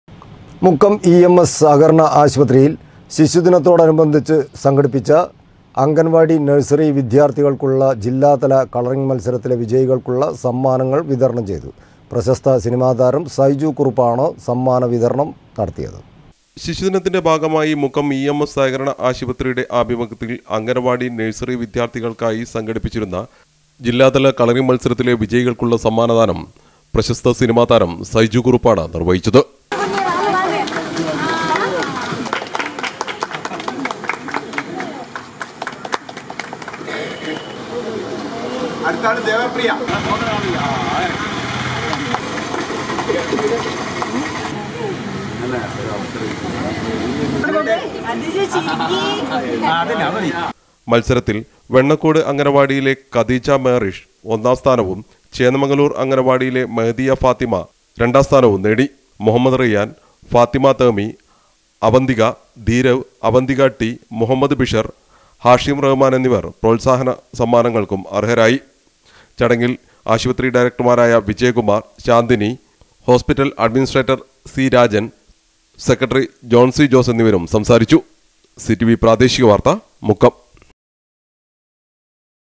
അംഗൻവാടി - നഴ്സറി വിദ്യാർത്ഥികൾക്കുള്ള ജില്ലാതല കളറിംഗ് മത്സരത്തിലെ വിജയി കൾക്ക് സമ്മാനങ്ങൾ വിതരണം
മുക്കം ഇ എം എസ് സഹകരണ ആശുപത്രിയിൽ ശിശുദിനത്തോടനുബന്ധിച്ച് സംഘടിപ്പിച്ച  അംഗൻവാടി - നഴ്സറി വിദ്യാർത്ഥികൾക്കുള്ള ജില്ലാതല കളറിംഗ് മത്സരത്തിലെ വിജയി കൾക്ക് സമ്മാനങ്ങൾ വിതരണം ചെയ്തു.